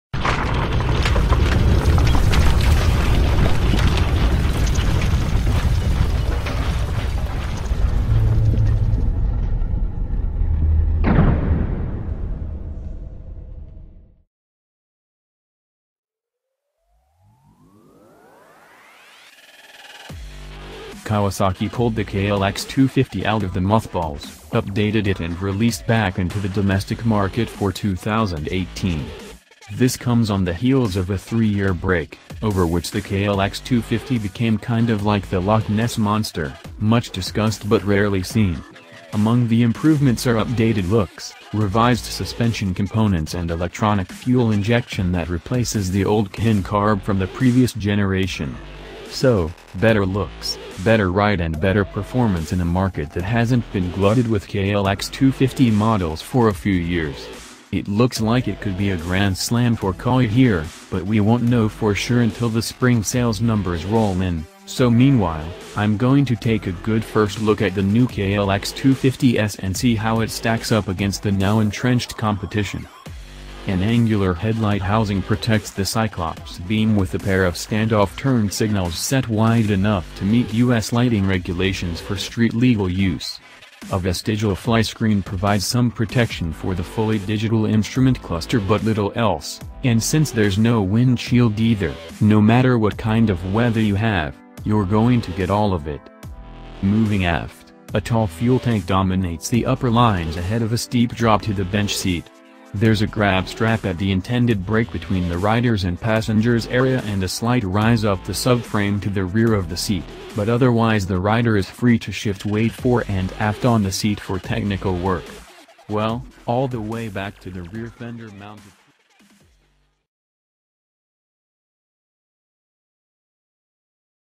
LOOK THIS !!!2018 Kawasaki Dtracker 250 White black
LOOK THIS !!!2018 Kawasaki Dtracker 250 White black - Kawasaki pulled the KLX250 out of the mothballs, updated it and released back into the domestic market for 2018. This comes on the heels of a three-year break, over which the KLX250 became kind of like the Loch Ness Monster, much discussed but rarely seen. Among the improvements are updated looks, revised suspension components and electronic fuel-injection that replaces the old Keihin carb from the previous generation. So, better looks, better ride and better performance in a market that hasn’t been glutted with KLX250 models for a few years. It looks like it could be a grand slam for Kawi here, but we won’t know for sure until the Spring sales numbers roll in, so meanwhile, I’m going to take a good first look at the new KLX250s and see how it stacks up against the now-entrenched competition.

██████ GET DAILY UPDATES! NOW! ██████